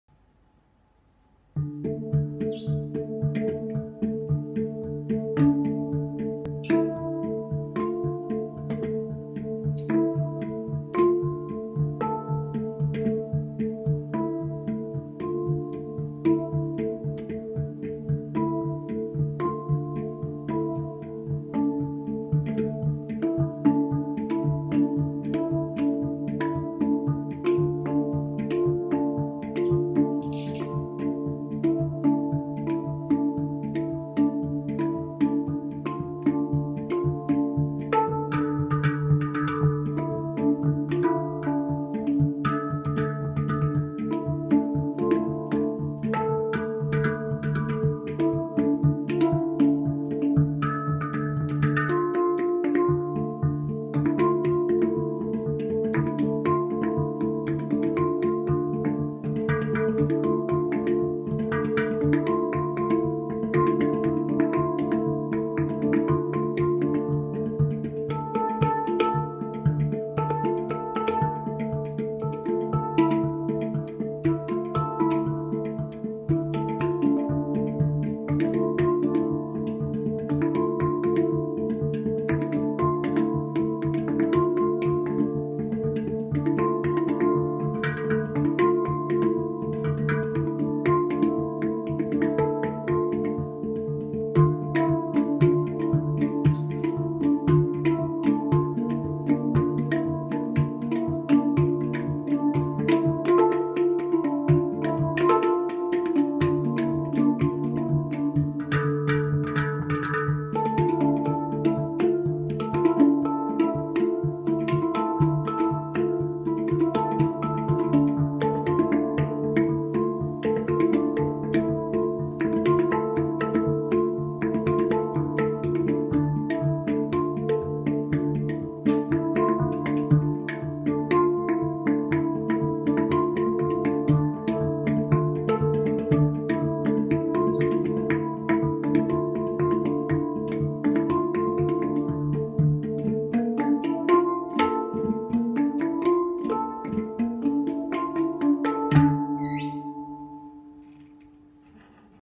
Hangdrum playing
Playing hang drum with my sister